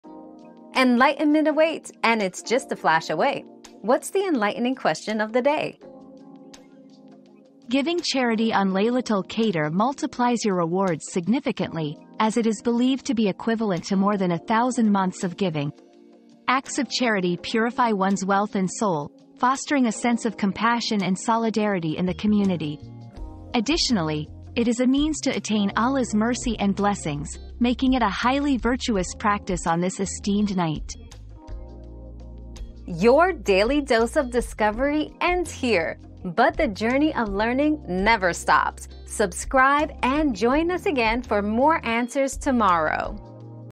What are the virtues of giving charity on Laylatul Qadr?
Unlocking Blessings: The Virtues of Charity on Laylatul Qadr 👉 Blessings of Charity 👉 Discover the profound rewards of giving charity on Laylatul Qadr, a night where deeds are multiplied exponentially. Purify wealth and soul, foster compassion, and bask in Allah's mercy with this virtuous act.

Laura S. Harris (2024, May 18.) What are the virtues of giving charity on Laylatul Qadr?
    🌐 AskAbout.video